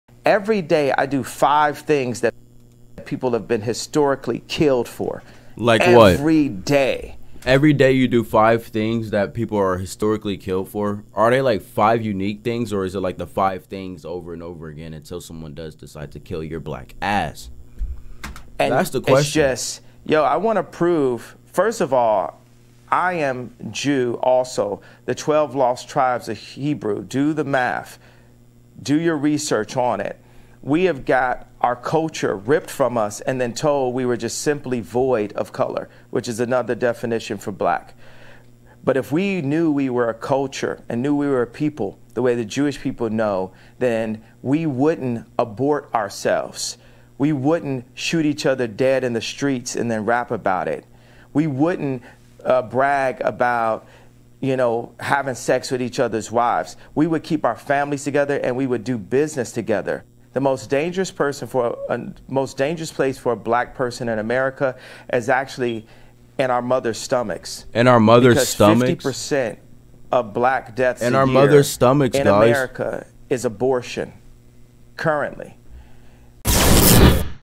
YYXOF Finds - PIERS MORGAN: KANYE "EVERYDAY I DO 5 THINGS THAT COULD GET ME KILLED" | Highlight #120
BELOW is a link to the full stream for the KANYE WEST and PIERS MORGAN full interview!

YYXOF Reacts to Ye Interview with Piers Morgan.

 I'm only reacting to it for educational and entertainment purposes.

I hope someone takes my stream just in case the interview or my stream is taken down indefinitely.

Kanye speaks volumes. . .

HOST: YYXOF